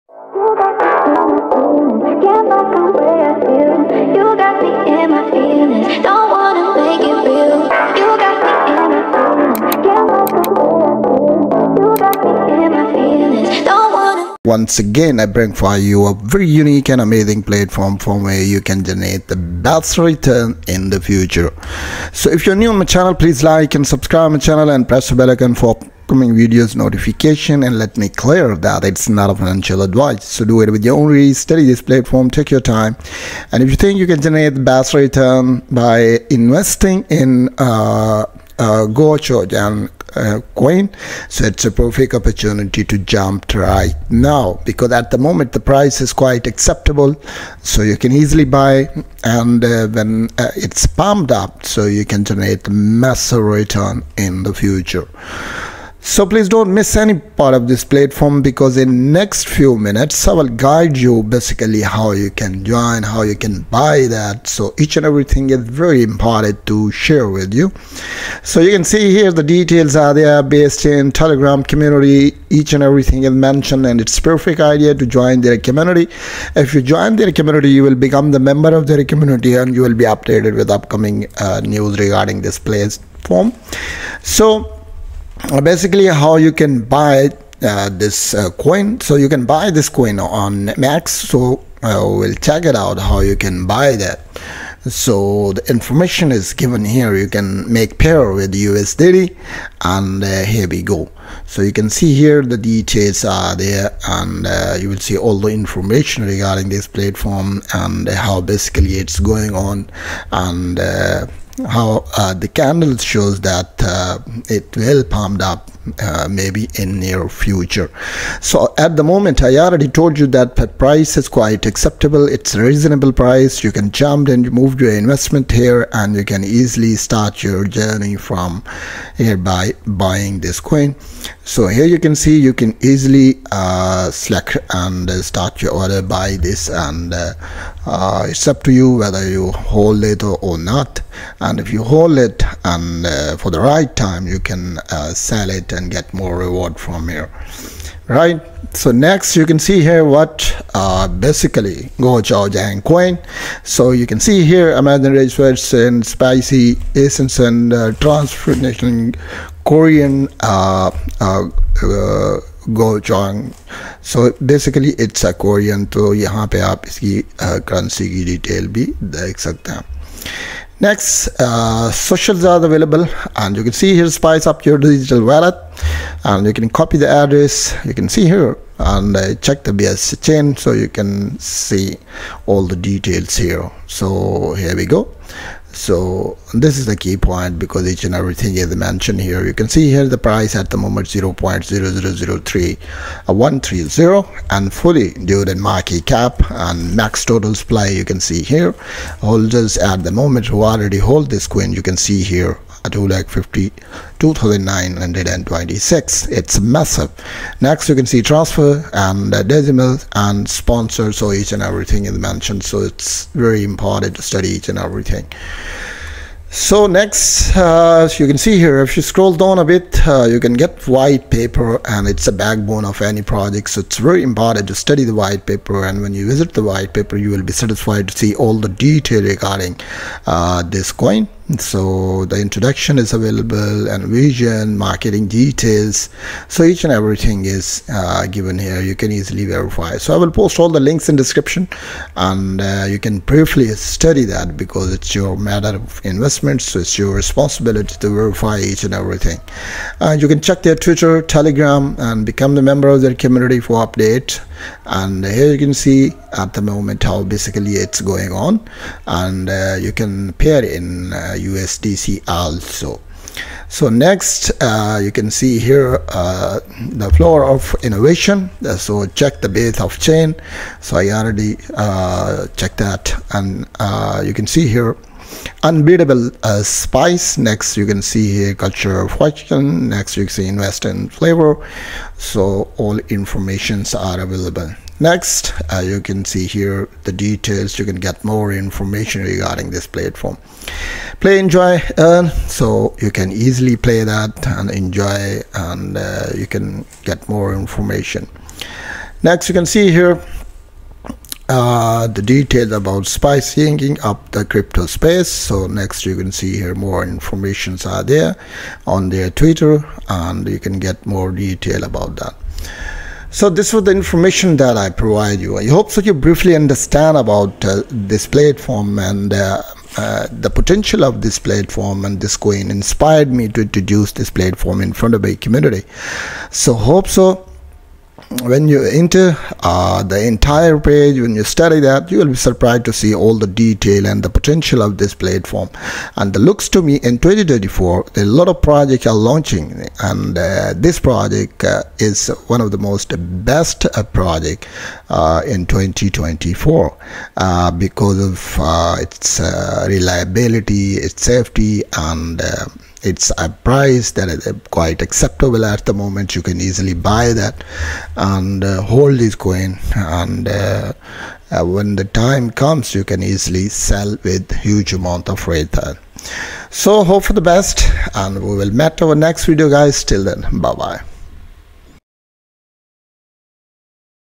GO CHU JANG COIN || GO CHU JANG THE SPICIEST CRYPTO IN THE MARKET || GET READY
What’s Gochujang Coin?
Imagine the rich, sweet, & spicy essence of traditional Korean Gochujang, reimagined as a digital currency. GochujangCoin is a token themed around Gochujang, which is one of the ingredients used in K-food cooking made from chili peppers. Although the memecoin ecosystem is already entertaining, $GOCHU is for everyone who appreciates healthy memetic communication. Whether you’re a fan of mouth-watering flavors or heart-pumping investments, GochujangCoin is here to add an extra kick to your day!

✅ Always check the correctness of the account spelling!